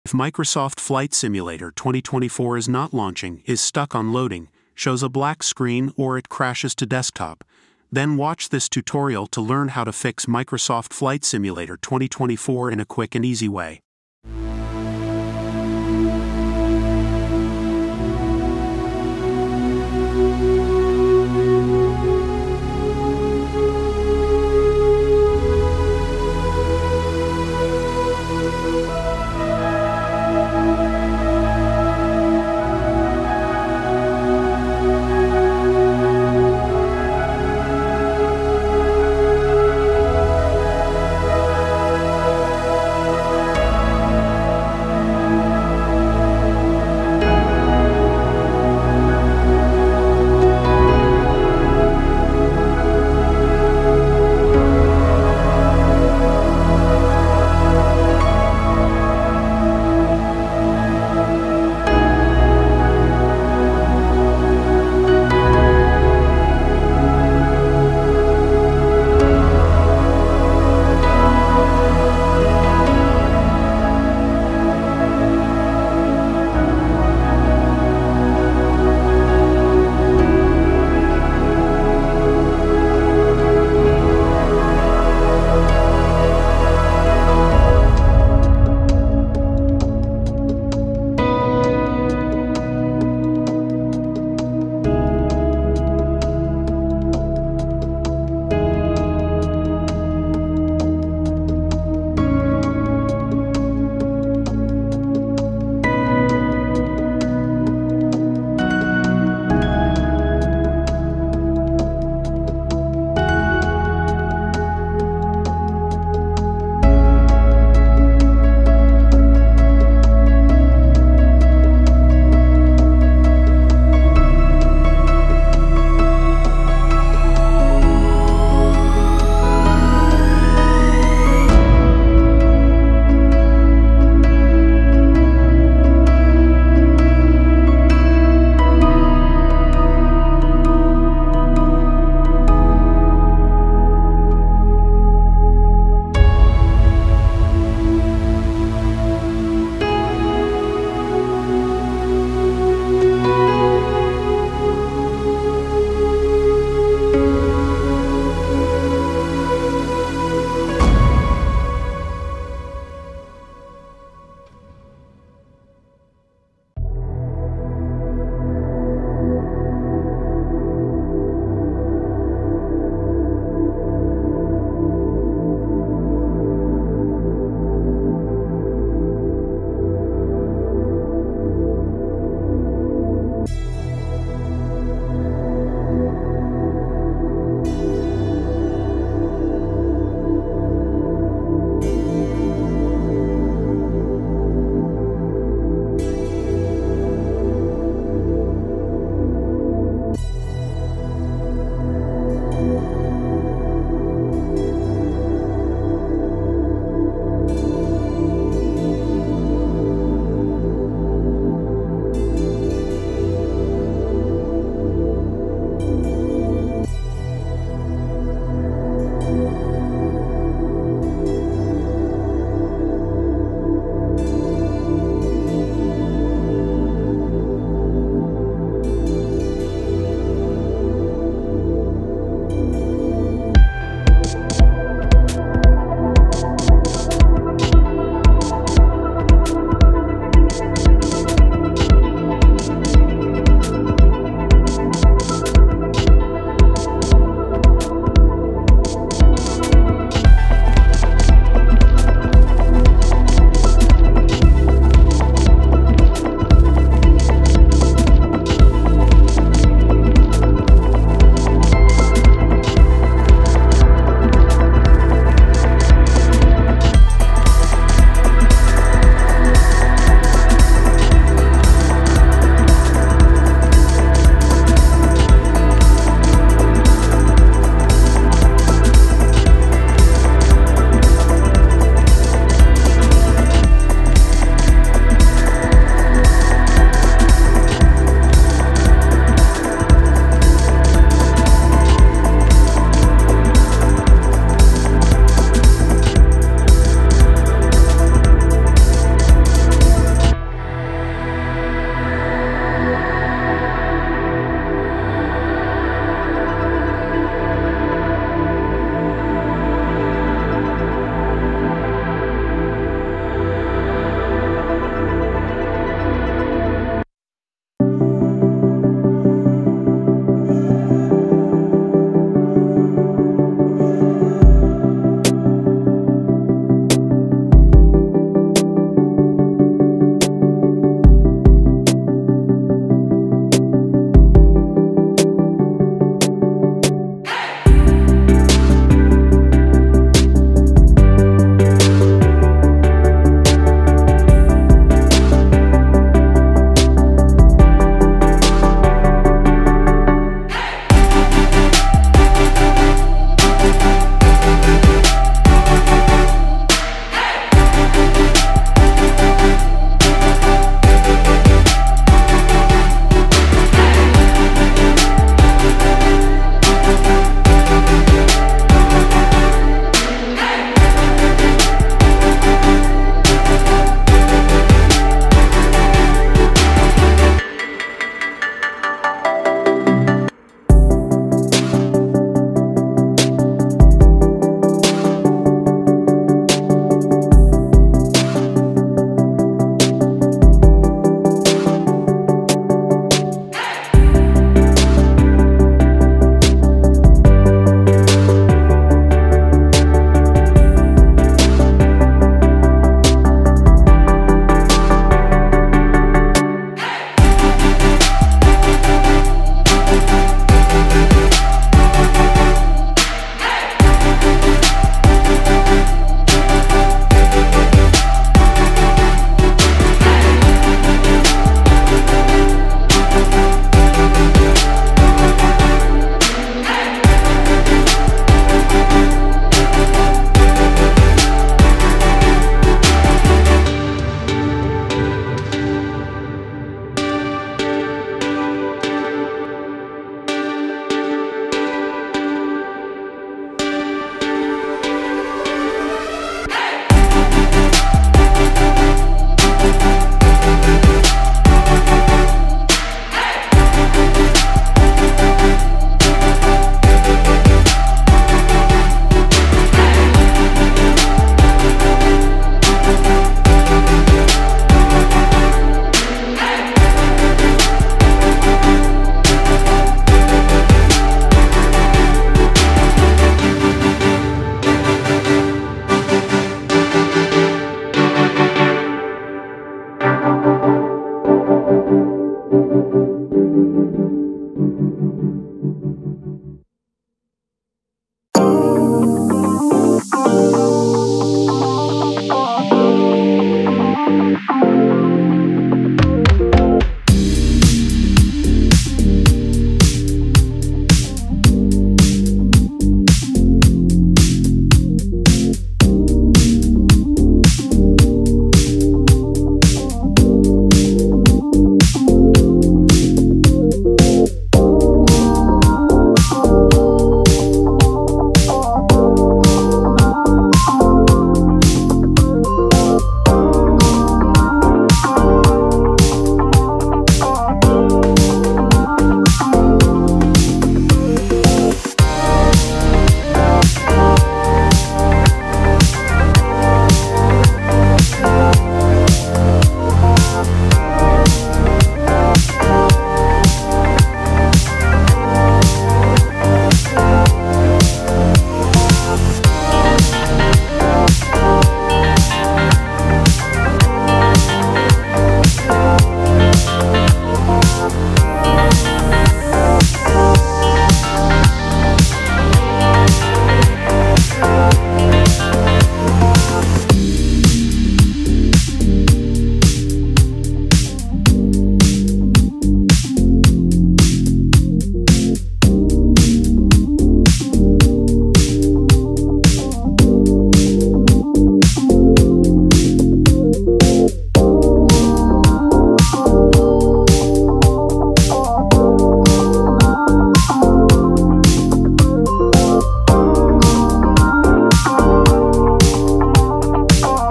Microsoft Flight Simulator 2026 Not Launching/Not Loading/Black Screen/Crash to Desktop on PC FIX
If Microsoft Flight Simulator 2026 is not launching, is stuck on loading, shows a black screen or it crashes to desktop. Then watch this tutorial to learn how to fix Microsoft Flight Simulator 2026 in a quick and easy way.Whether you're encountering sudden crashes, launch failures, or mysterious glitches, we've got the solutions you need. Join us as we explore potential culprits, share practical troubleshooting tips, and guide you through the step-by-step process of getting "Microsoft Flight Simulator 2026" up and running seamlessly on your PC.

CHAPTERS
00:00 Intro
00:15 Solution 1
01:24 Solution 2
02:40 Solution 3
04:30 Solution 4
06:54 Solution 5
09:51 Outro